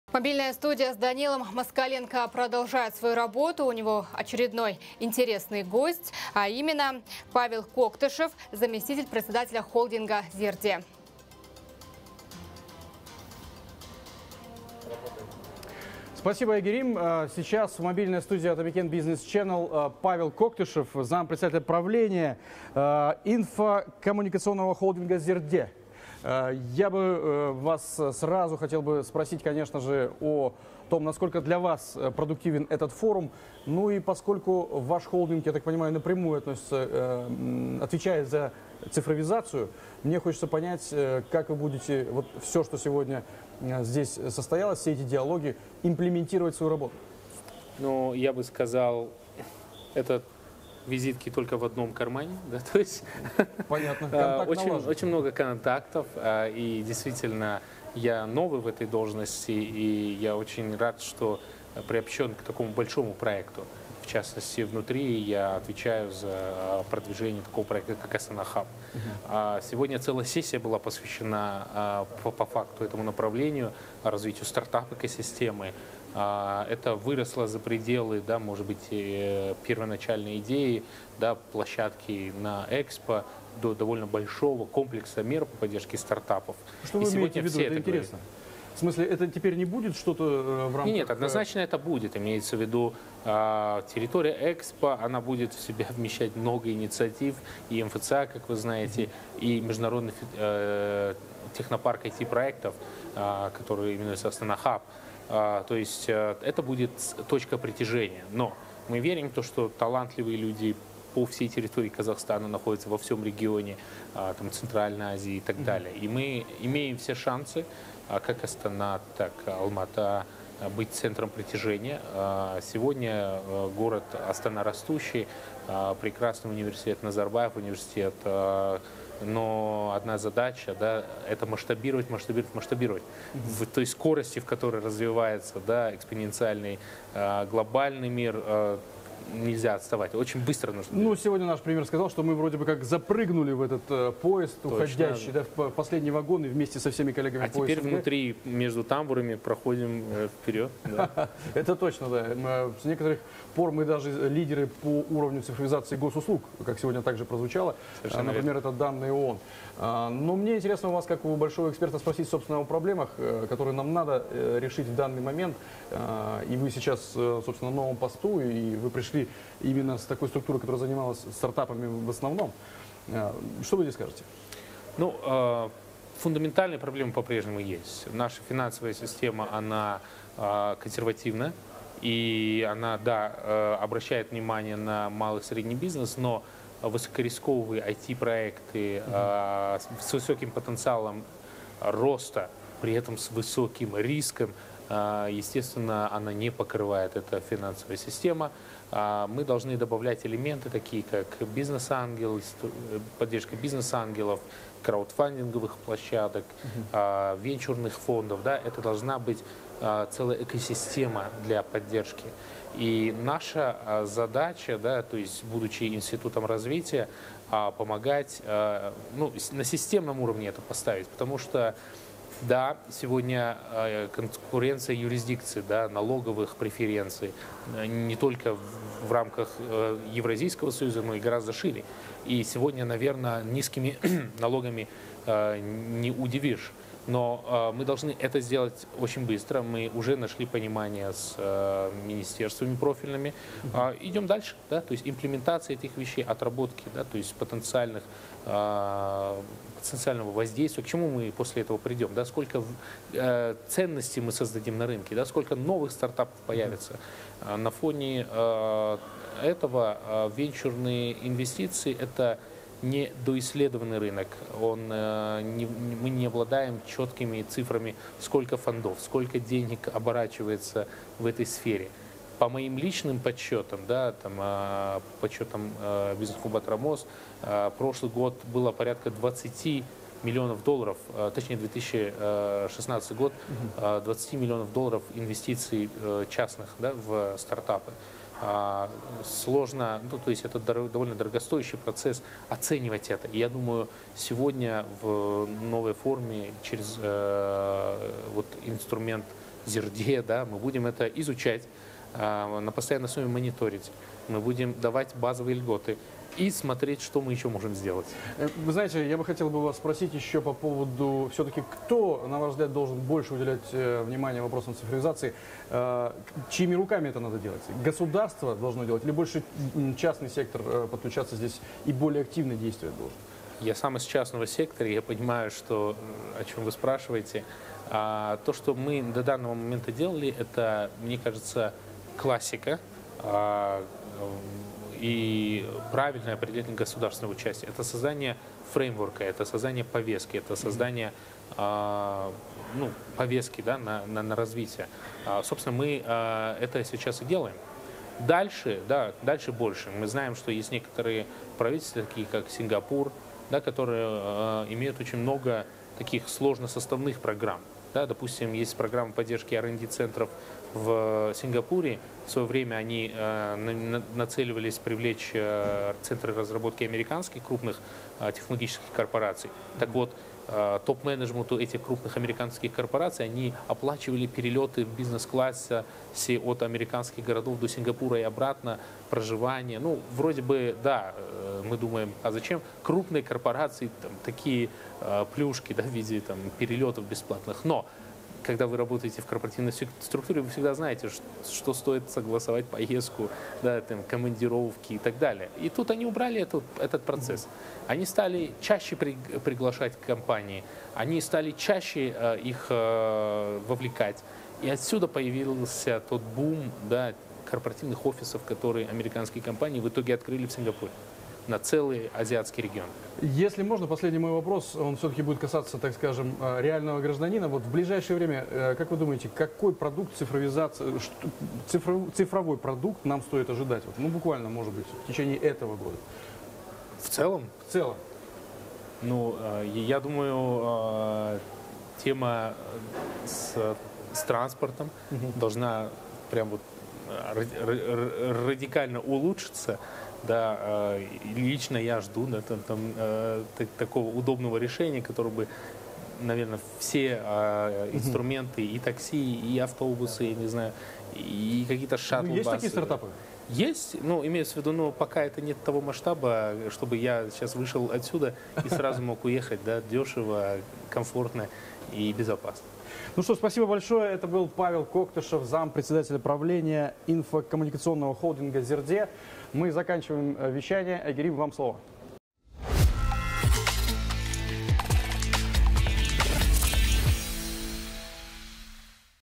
Интервью с зампредом "Зерде" Павлом Коктышевым (02.02.2018)
Тема: "Цифровая повестка в эпоху глобализации". Гость беседы: заместитель председателя правления Национального инфокоммуникационного холдинга "Зерде" - Павел Коктышев. Ведущий: Данил Москаленко